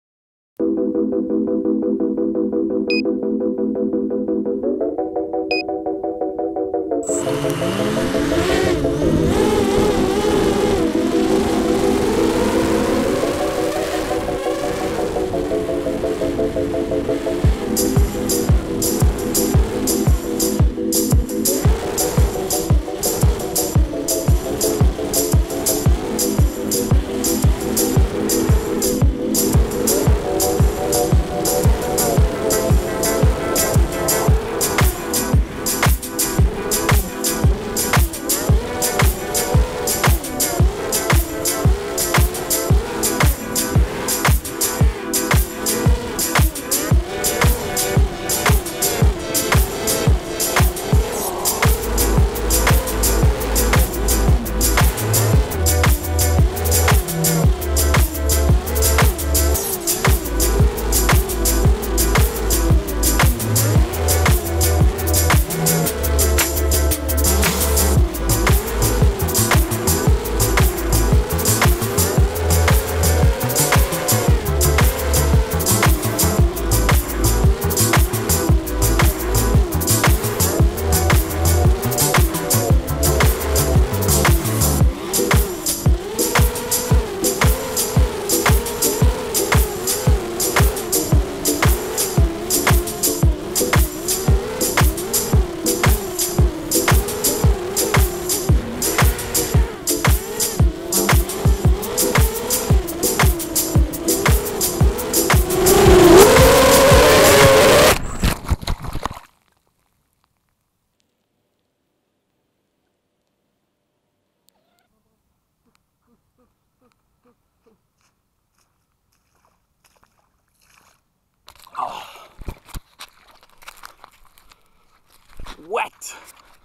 FPV - Krieger
First video with the Krieger

Quad parts:
Krieger 225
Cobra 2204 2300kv
RG20s
ImmersionRC Raceband 5.8GHz 200mW Transmitter
ImmersionRC Uno 5.8GHz Receiver
5.8GHz SpiroNET Mini Patch Antenna
fatshark predator v2
DAL 5045 Bullnose props
1300mah 4s
FrSky Taranis with X4R receiver running S.Bus
mobius